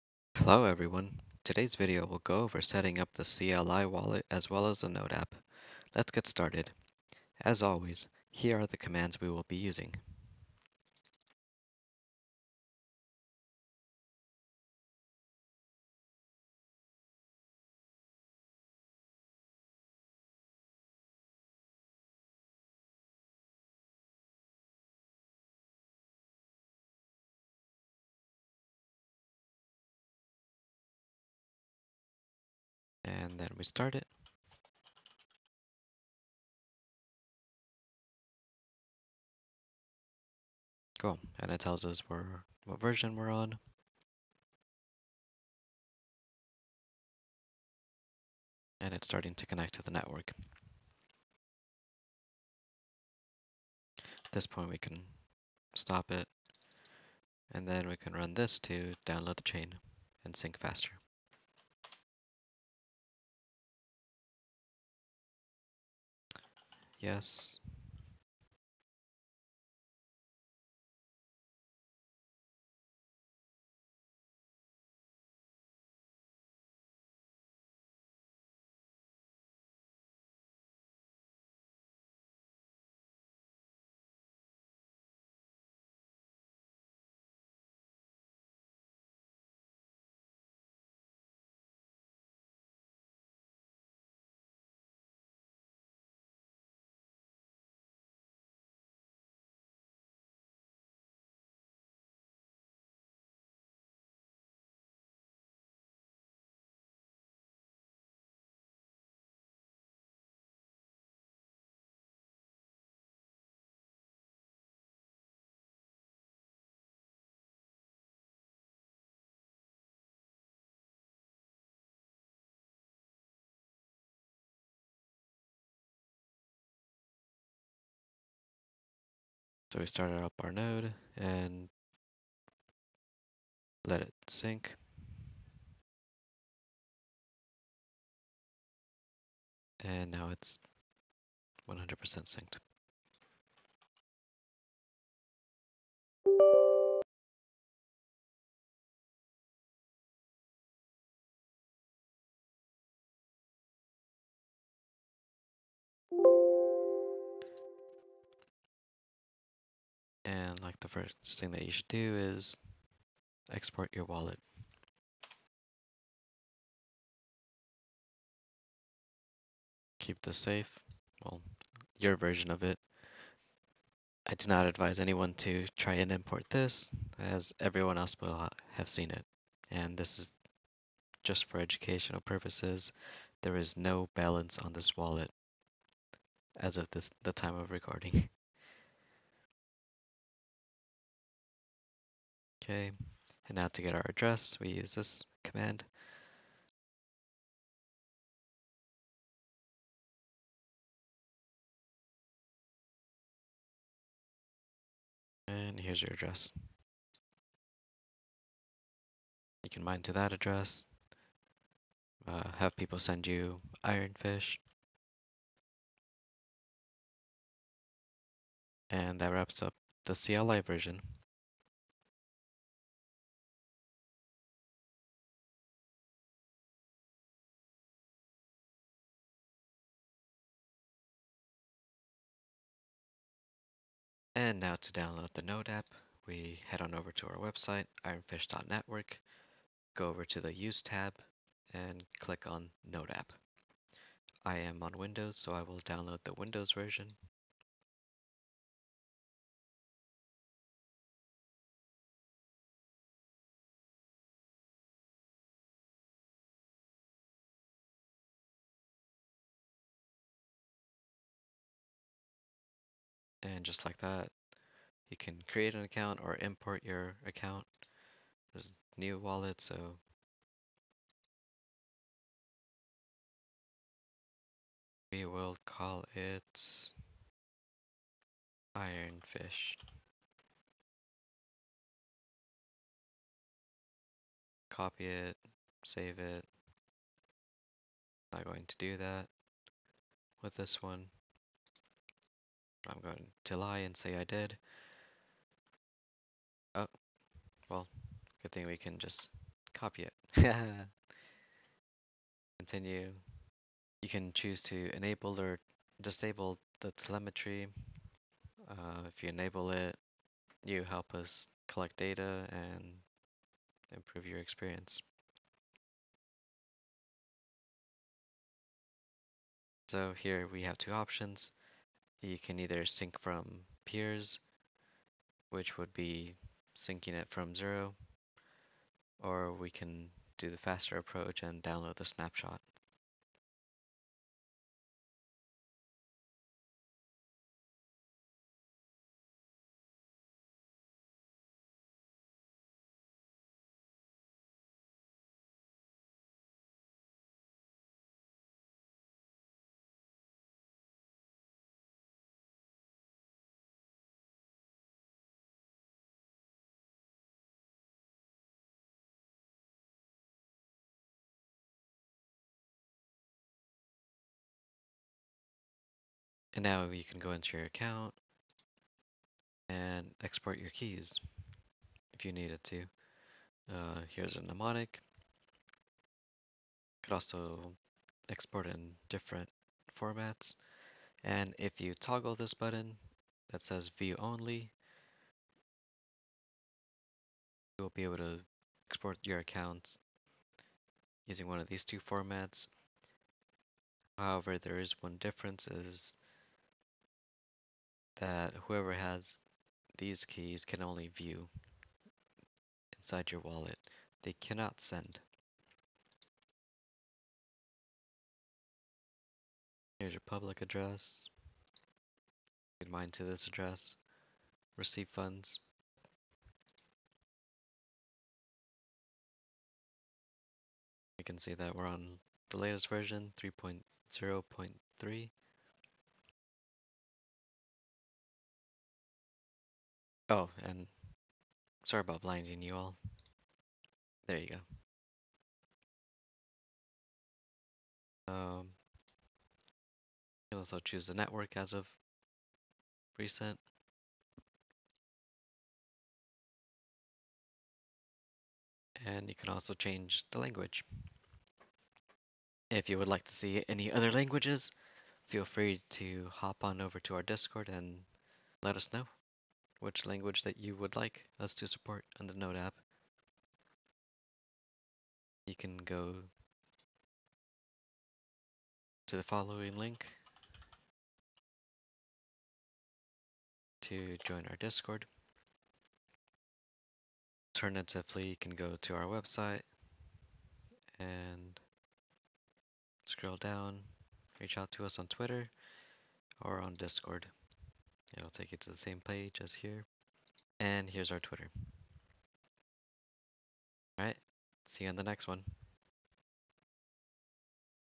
Complete Walkthrough on How to Setup an Iron Fish Wallet! Both CLI and Node App!
This video serves as an educational walkthrough on how to setup the CLI node wallet as well as the Node App. The CLI is text based while the Node App is a graphical user interface.